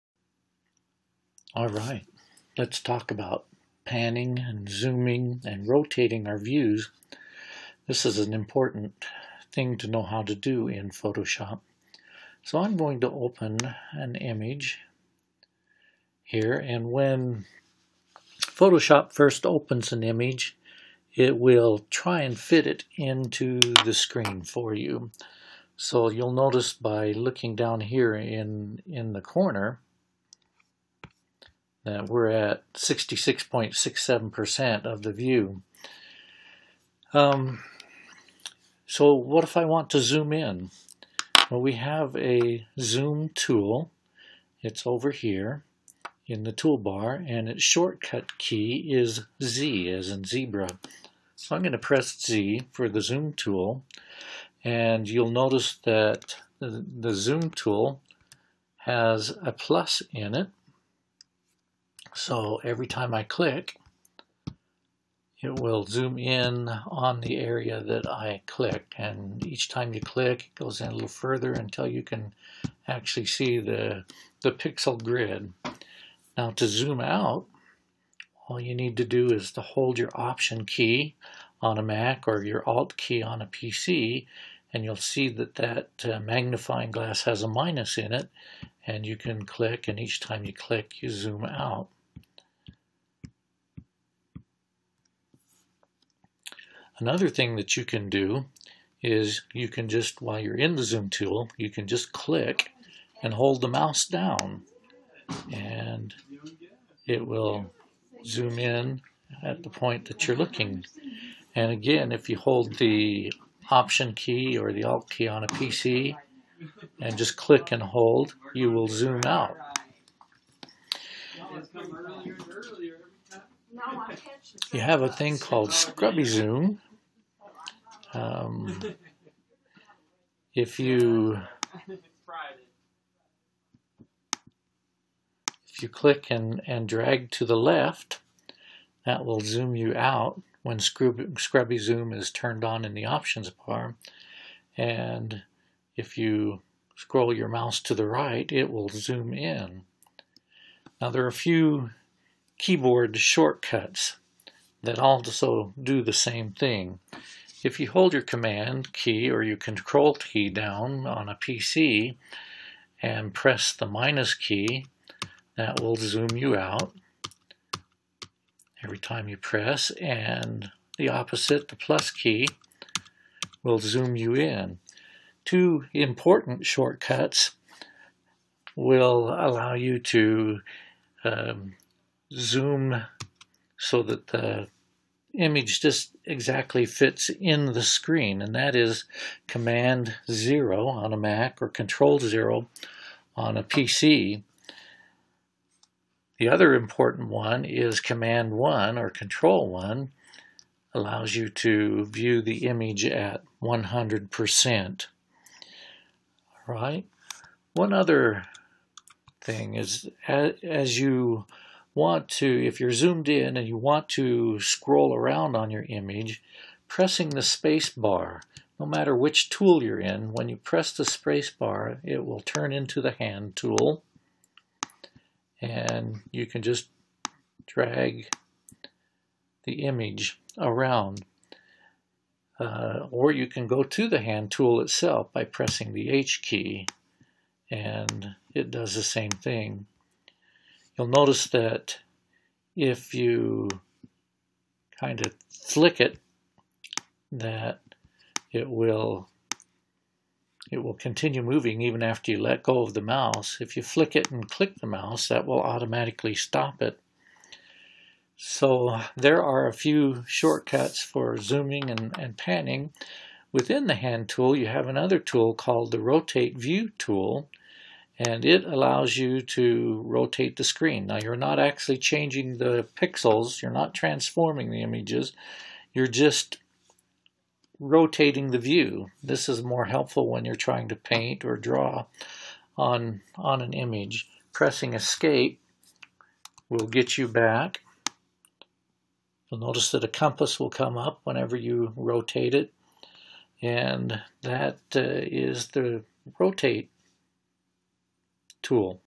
Photoshop: Panning, Zooming, Rotating Views
This video demonstrates multiple ways to zoom in or out of your image in Photoshop.  It also show how to rotate the image to make it easier to draw or paint on.